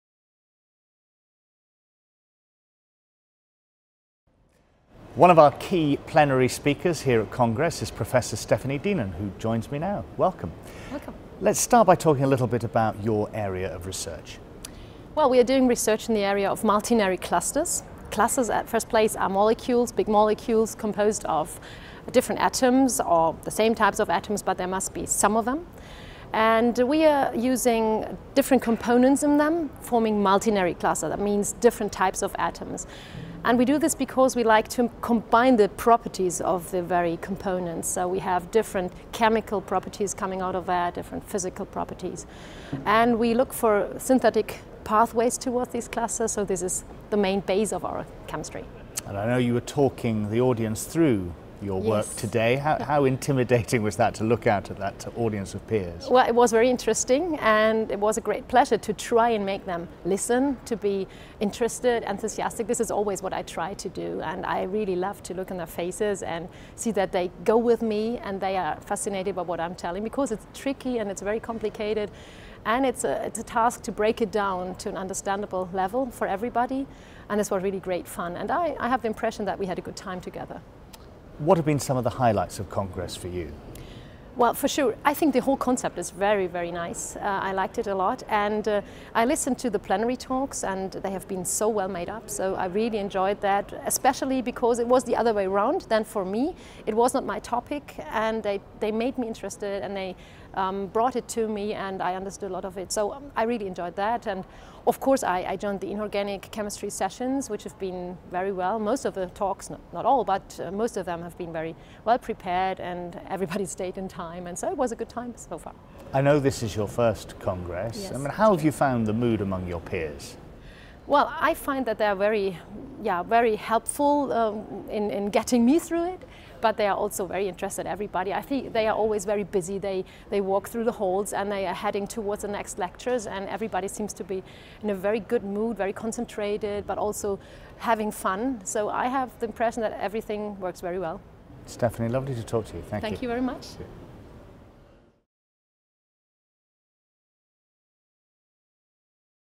Professor Stefanie Dehnen, EuChemS Congress plenary speaker
Professor Stefanie Dehnen, from the Philipps University of Marburg, talks us through her research into multinary clusters and what it's been like to deliver a plenary lecture at the 7th EuChemS Chemistry Congress in Liverpool.